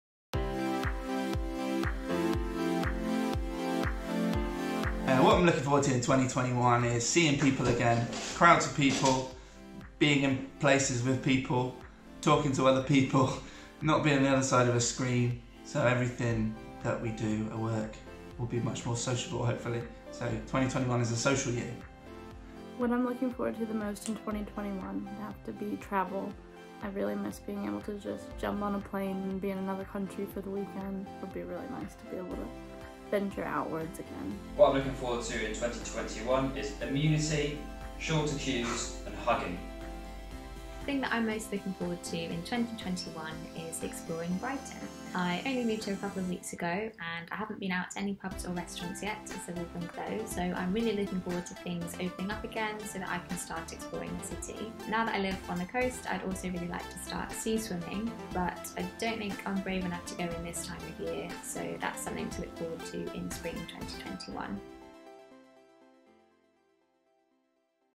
What are you most looking forward to in 2021? | Humans at Huxley
Dom, Chelsey, Tom, and Hannah talk about what they're looking forward to in 2021.

Part of a monthly series, Humans at Huxley, where we prove we're actually human!

Huxley Digital is an accessible web design and development agency based in Worthing, UK.